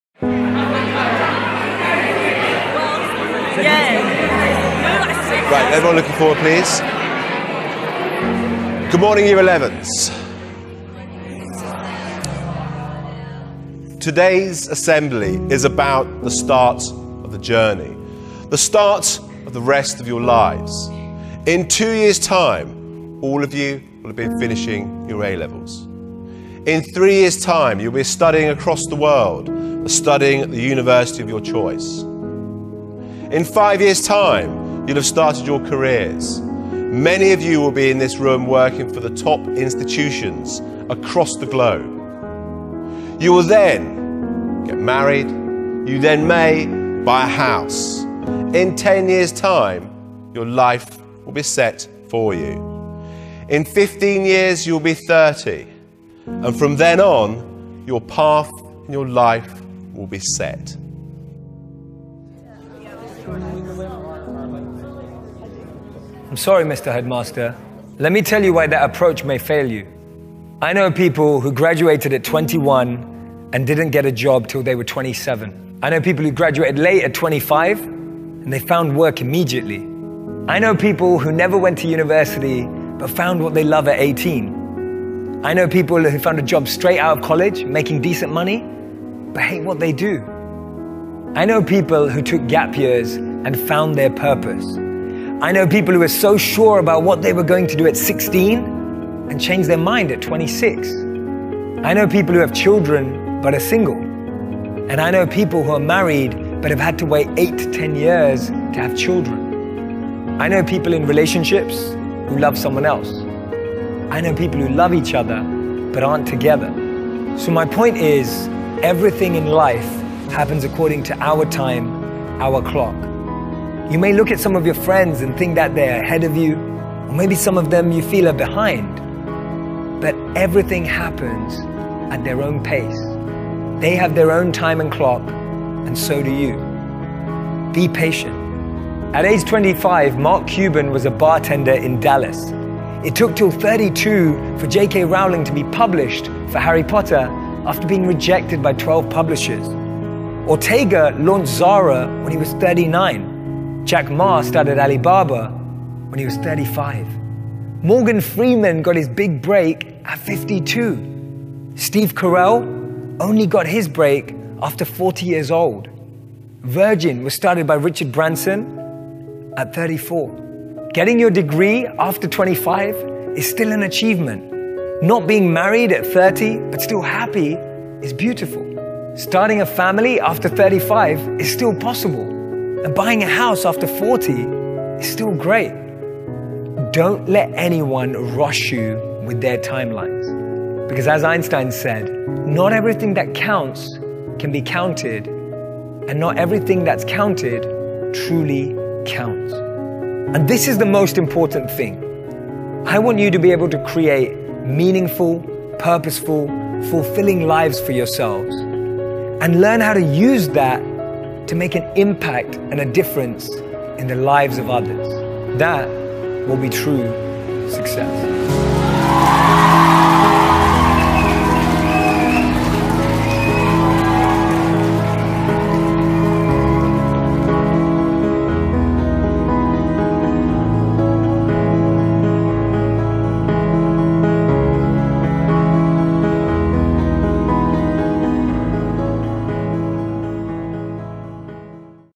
Sucesso?
Você acha que está “atrasado” na vida?
Ou que está à frente dos outros?
Assista ao vídeo e me diga se ainda pensa assim...